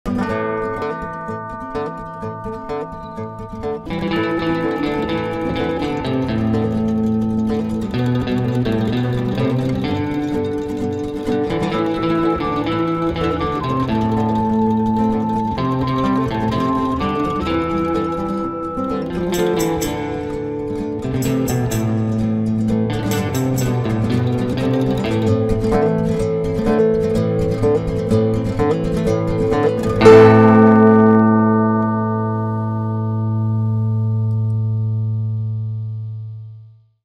Killer Comanche - Issue 2 - Teaser
Blue Feather's past comes back to haunt him as he seeks revenge on those who murdered an old friend.